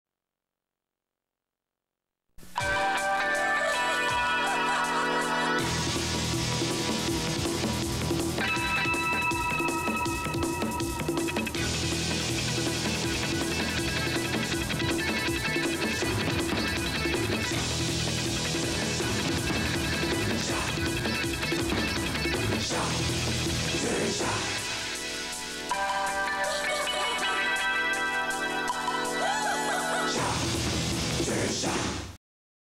Jovan Musk "What Is Sexy?" 1987 TV commercial
Jovan Musk "What Is Sexy?" 1987 TV commercial. The BetaGems channel also has "Jovan 1988 What is sexy? TV commercial."